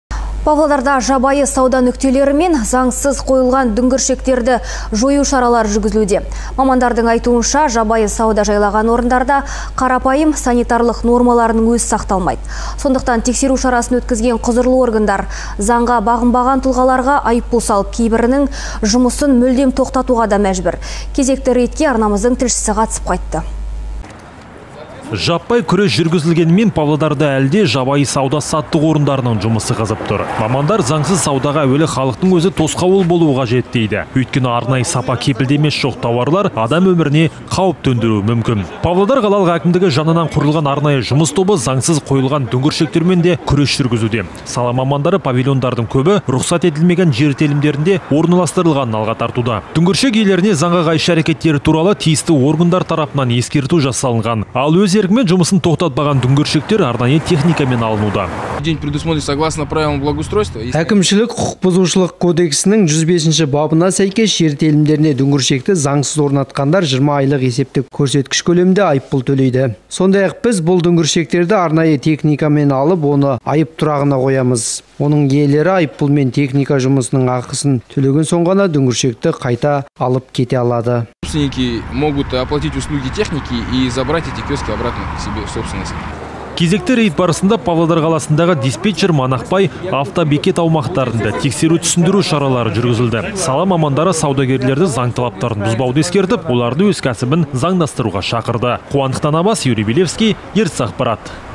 ЗАҢСЫЗ ДҮҢГІРШЕКТЕРМЕН КҮРЕС ЖҮРГІЗІЛУДЕ
Павлодарда жабайы сауда нүктелері мен заңсыз қойылған дүңгіршектерге қарсы күрес жүргізілуде. Мамандардың айтуынша, жабайы сауда жайлаған орындарда қарапайым санитарлық нормаларының өзі сақталмайды.  Сондықтан тексеру шарасын  өткізген  құзырлы органдар заңға бағынбаған тұлғаларға айыппұл салып, кейбірінің жұмысын мүлдем тоқтатуға да мәжбүр. Кезекті рейдке арнамыздың тілшісі қатысып қайтты. Жаппай күрес жүргізілгенімен Павлодарда әлі де жабайы сауда-саттық орындарының жұмысы қызып тұр. Мамандар заңсыз саудаға әуелі халықтың өзі тосқауыл болуы қажет дейді. Өйткені, арнайы сапа кепілдемесі жоқ тауарлар адам өміріне қауіп төндіруі мүмкін. Павлодар қалалық әкімдігі жанынан құрылған арнайы жұмыс тобы заңсыз қойылған дүңгіршектермен де күрес жүргізуде. Сала мамандары павильондардың көбі рұқсат етілмеген жер телімдерінде орналастырылғанын алға тартуда. Дүңгіршек иелеріне заңға қайшы  әркеттері туралы тиісті органдар тарапынан ескерту  жасалынған. Ал өз еркімен жұмысын тоқтатпаған дүңгіршектер арнайы техникамен алынуда.